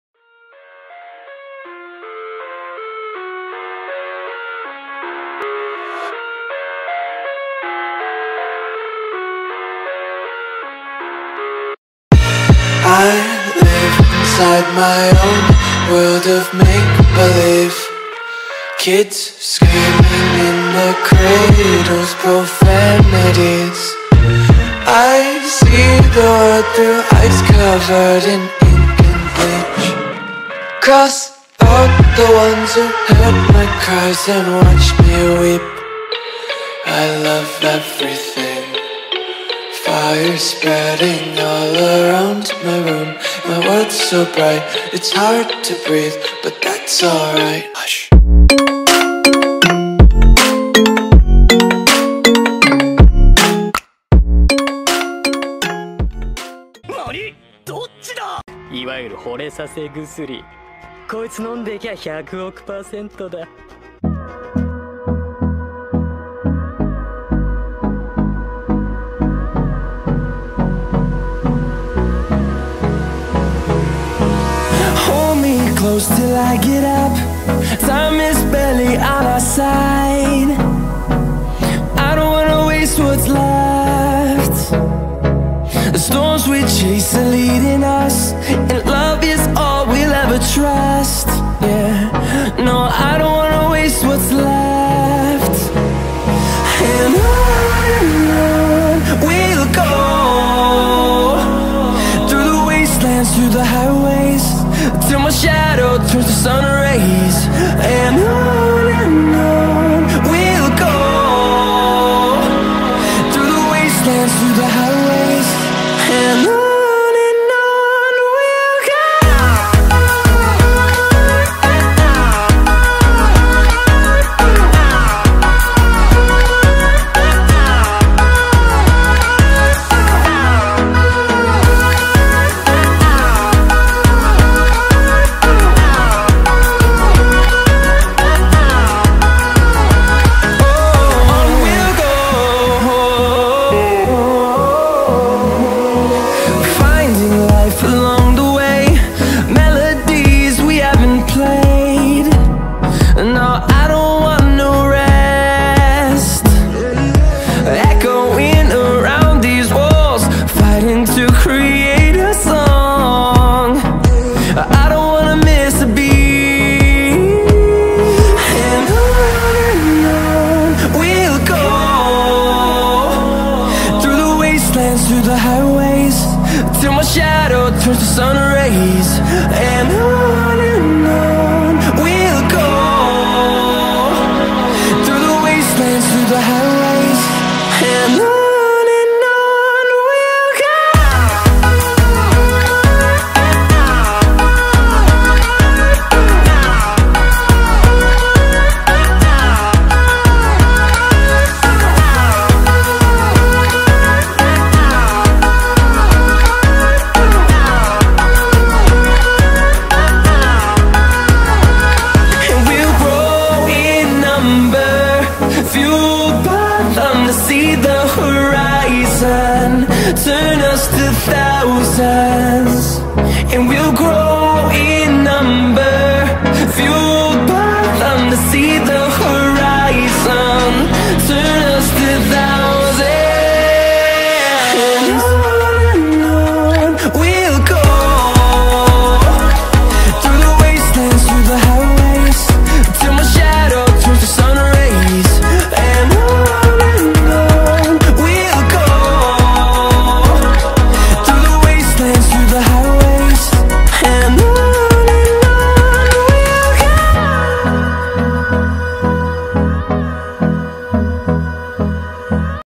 Drawing Senku | Speed Drawing | Anime Drawing | Dr.Stone
A speed Drawing timelapse of senku! Senku is the main character from the popular anime "Dr.Stone". Enjoy!